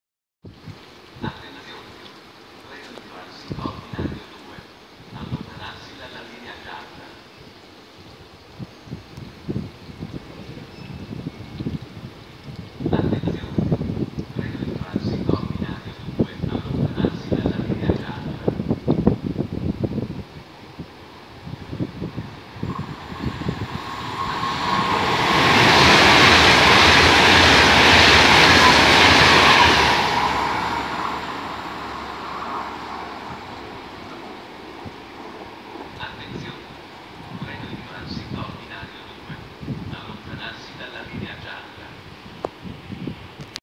Monselice, stazione di Monselice(9)
1)R20476 14:39 Monselice Mantova 4
2)Transito Frecciarossa 1000
3)Transito merci container Nacco Veronesi con in testa locomotore E652.156
4)Transito Frecciarossa
5)Transito Frecciargento
Video girati il 9/08/2017